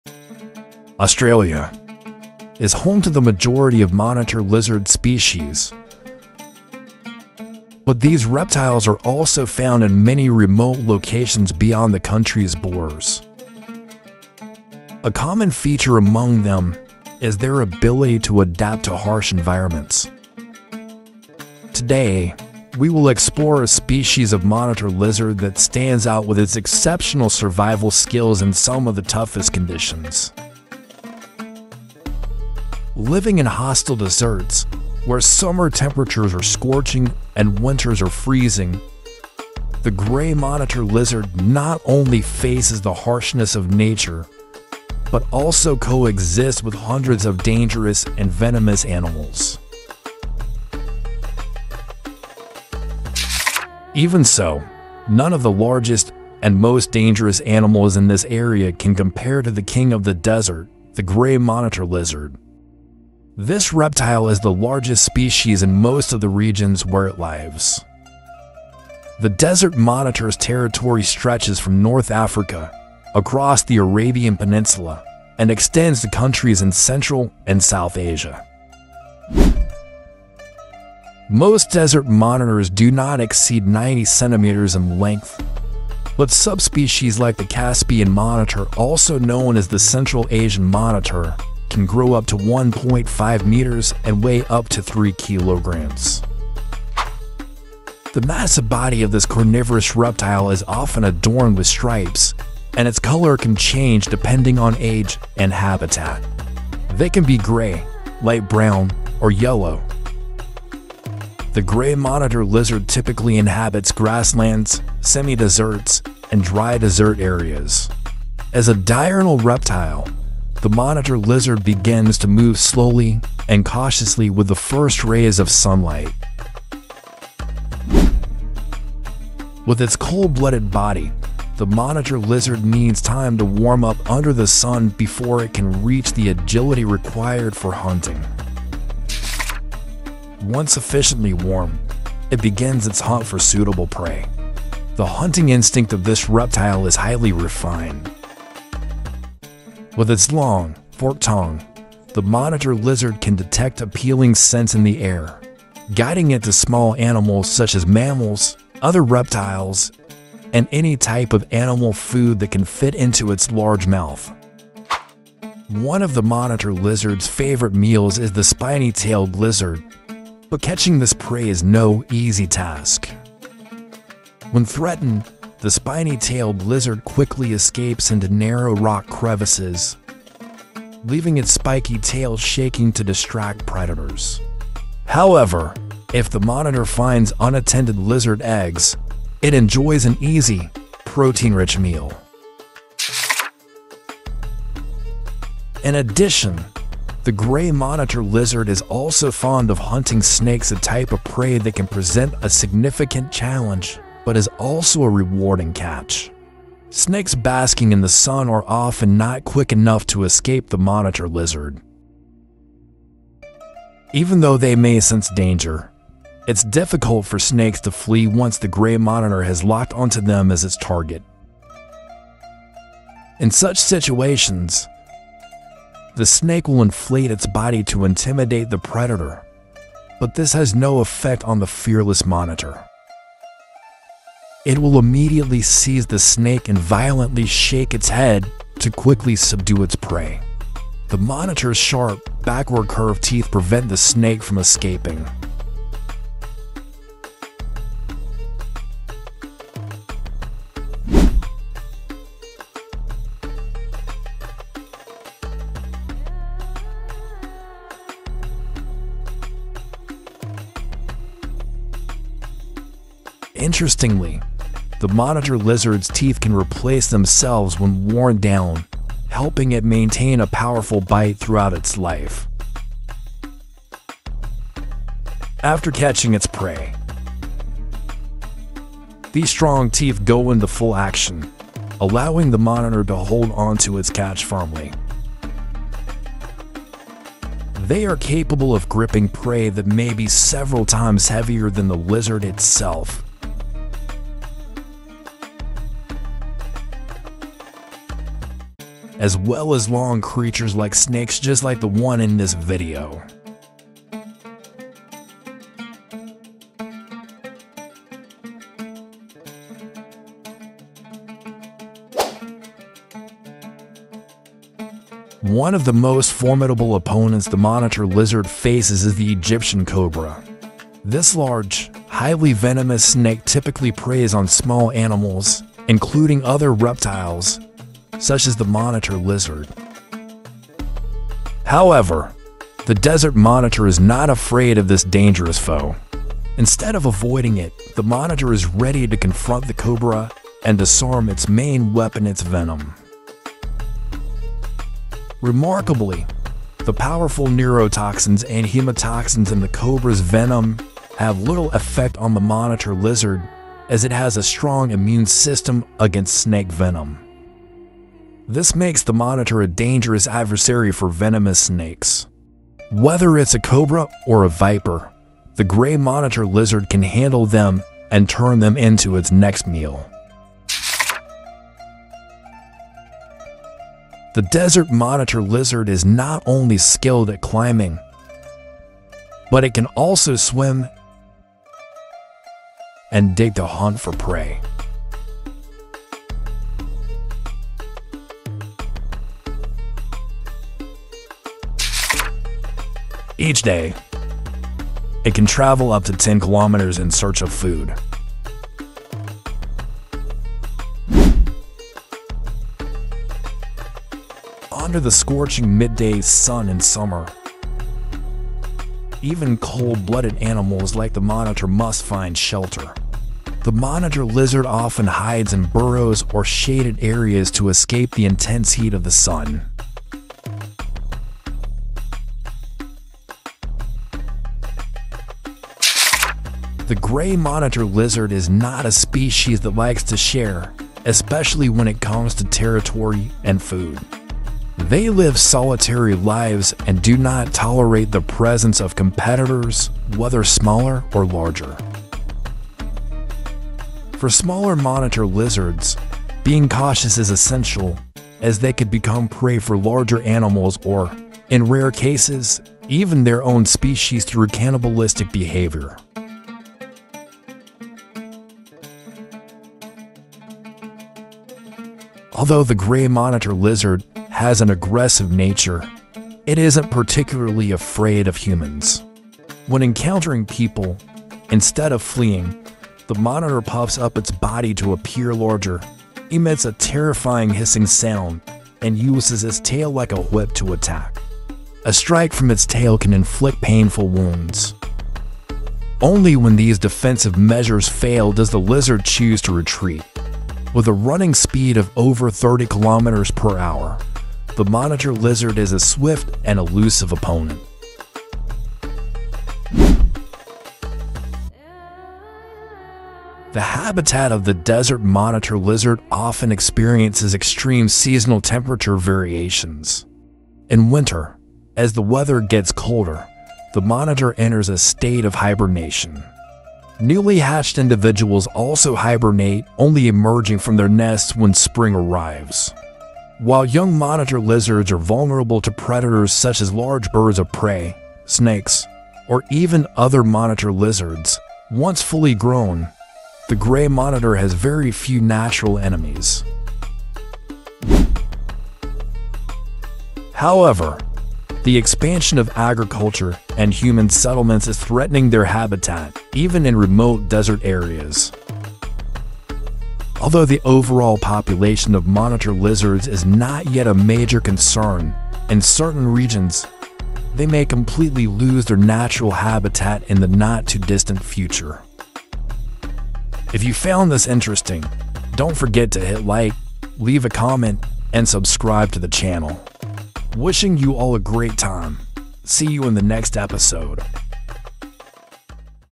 The King of the Desert: Gray Monitor Lizard Survival in Harsh Environments
Explore the fascinating world of the gray monitor lizard, the king of the desert! 🦎 Learn how this amazing reptile adapts to survive in some of the harshest environments on Earth, from the scorching heat of the desert to the freezing winters. 🌞❄️

In this video, we uncover:

The hunting techniques of the gray monitor, including how it catches its favorite prey like the spiny-tailed agama and venomous snakes like the Egyptian cobra. 🐍
How its immunity to snake venom makes it a deadly predator, even against the most dangerous snakes!
Its incredible survival skills, including climbing, swimming, and digging to find food, and how it travels up to 10 km a day in search of prey.
The seasonal hibernation habits of this monitor lizard and how it withstands extreme temperatures.
The threats posed by human development and how this resilient species continues to thrive in shrinking habitats.
If you love learning about wildlife, reptiles, and desert survival, this video is for you! 👍